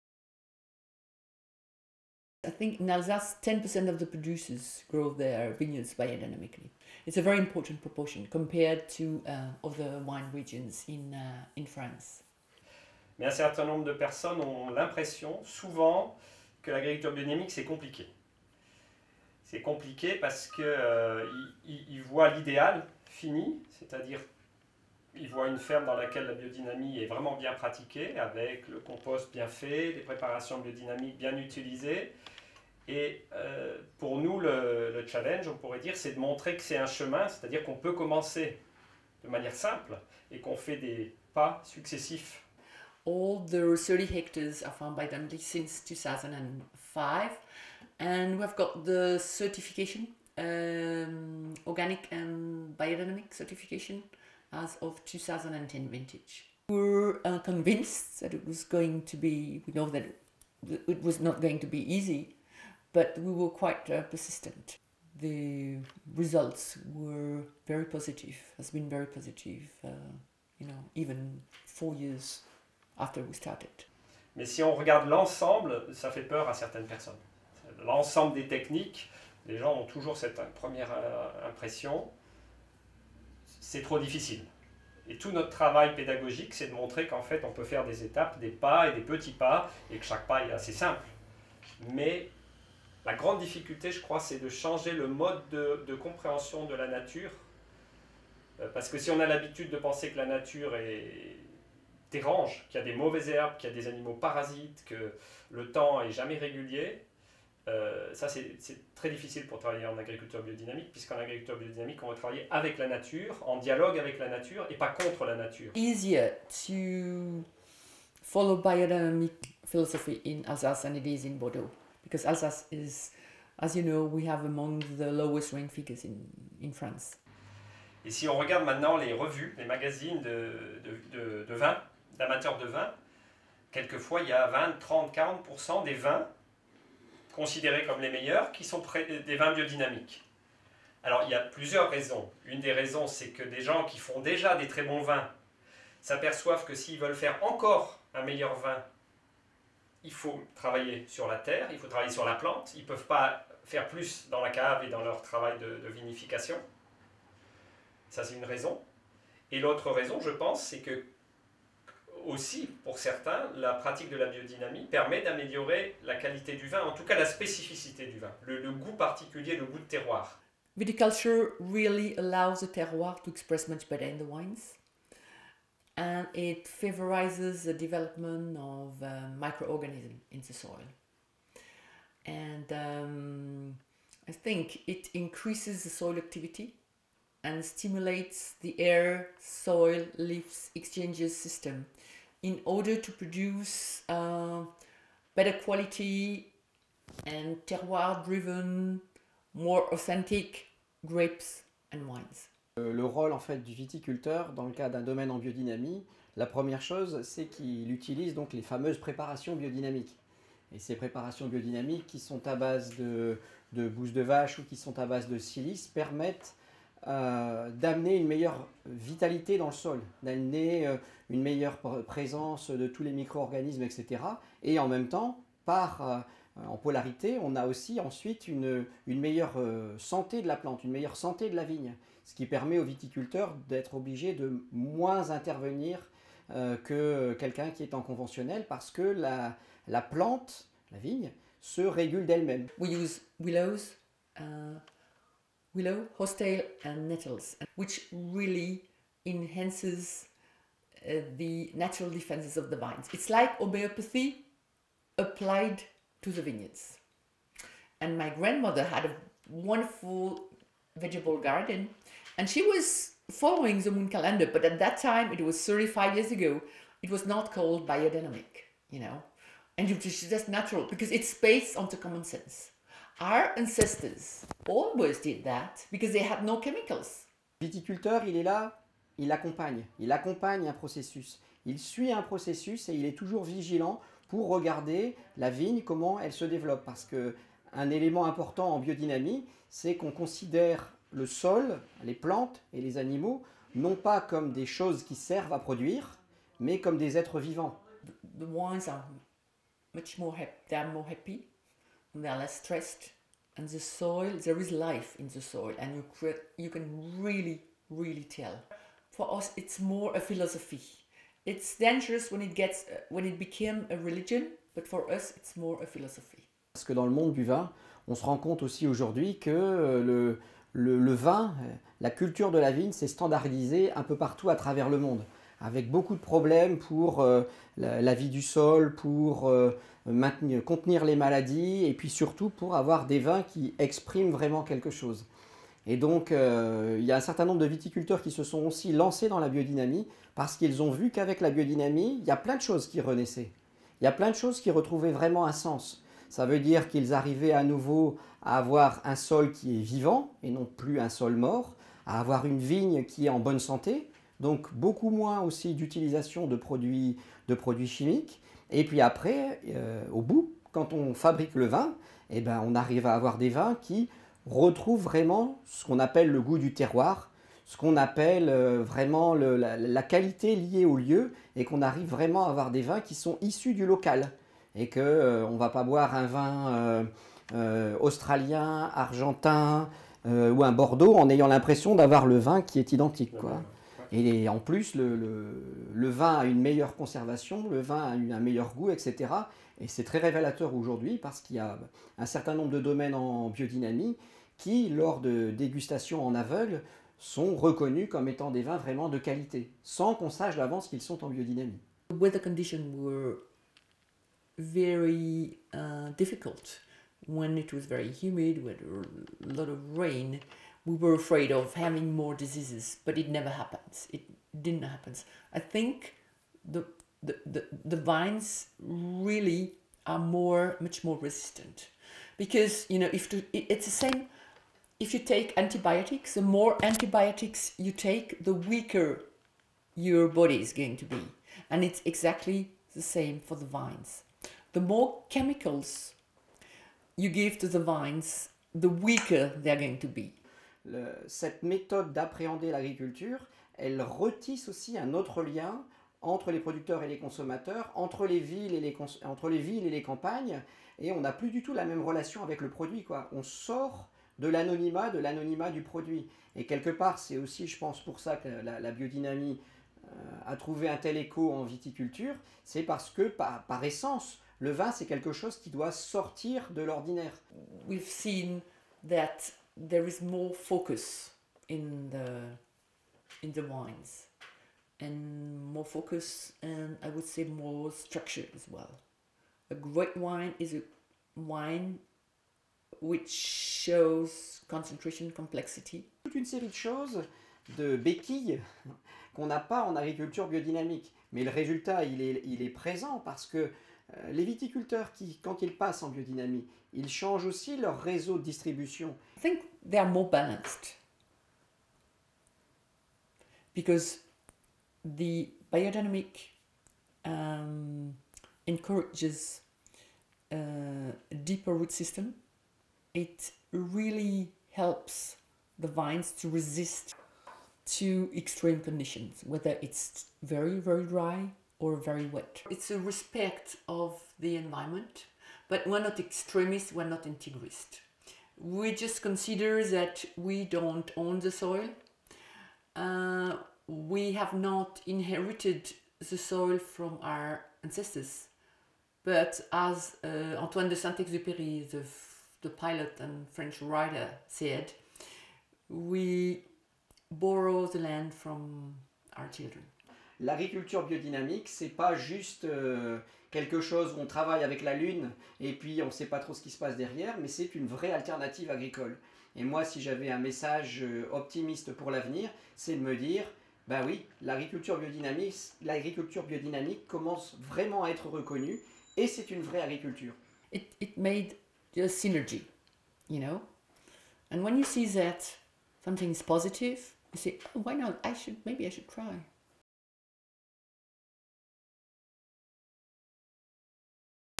Biodynamic Wine Production in Alsace
Catherine Faller of Domaine Weinbach, a W&S Top 100 Winery for 2013, speaks out on biodynamic winegrowing in Alsace.